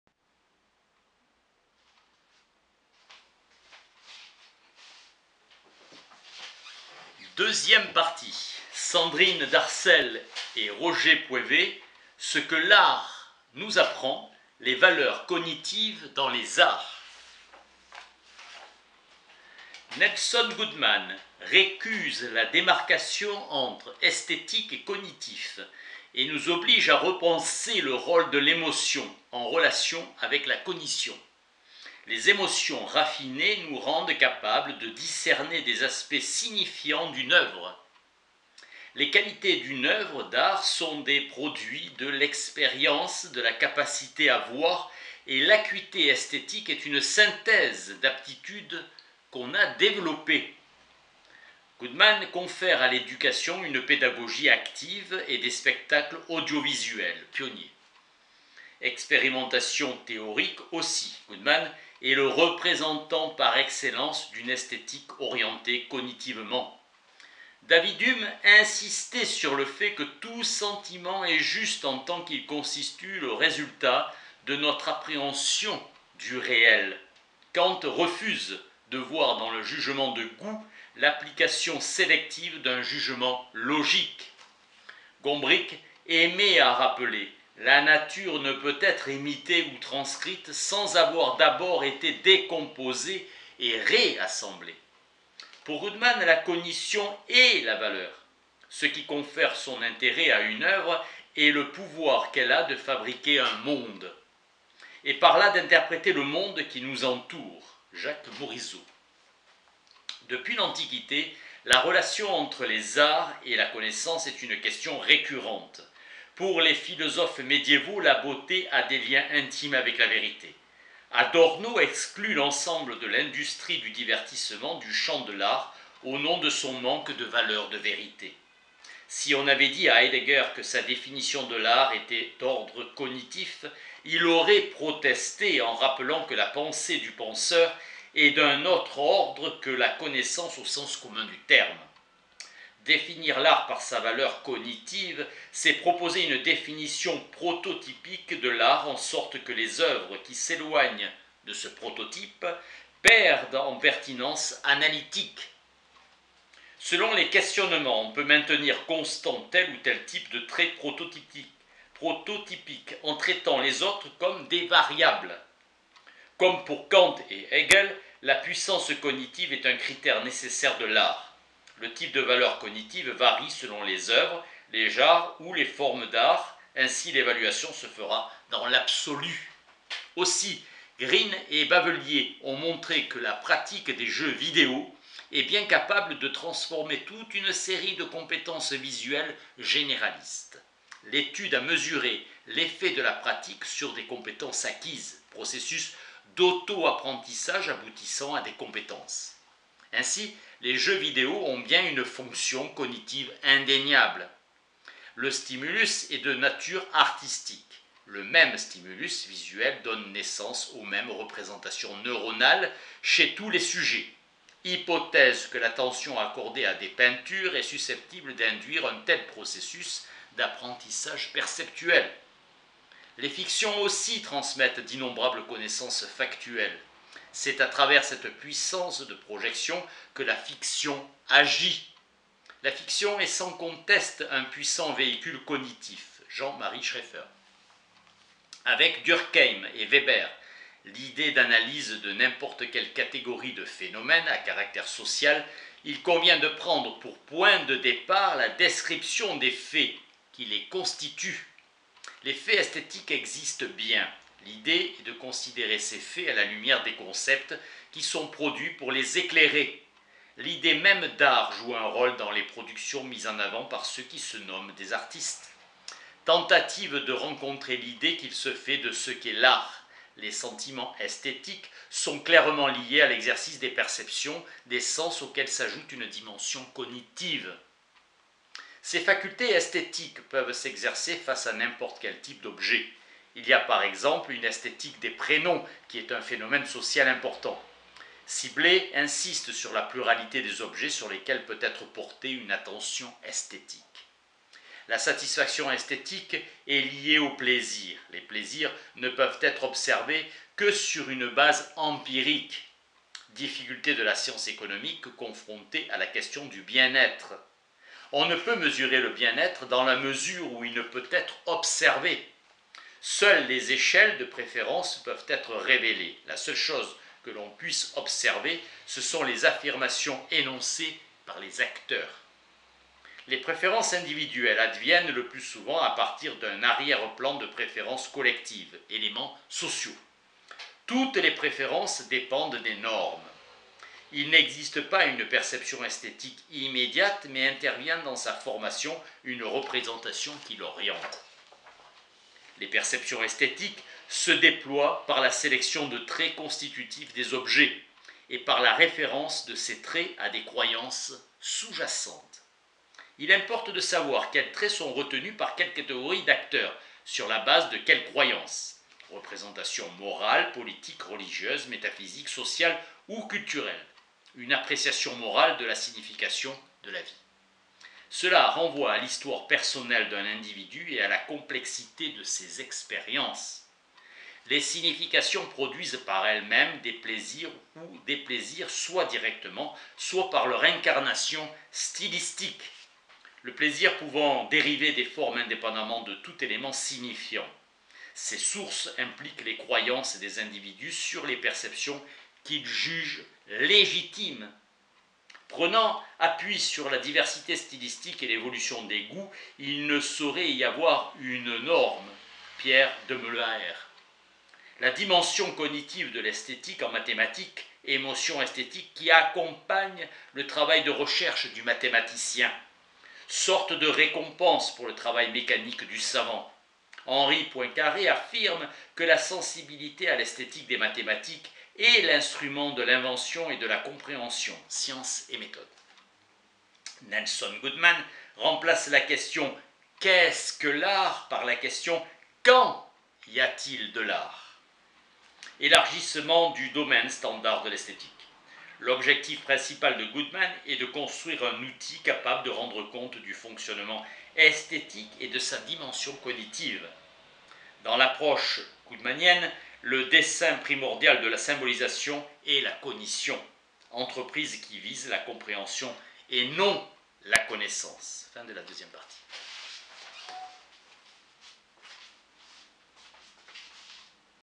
Sandrine Darsel et Roger Pouivet: Ce que l'art nous apprend. 2ième partie.
Les valeurs cognitives dans les arts 2/3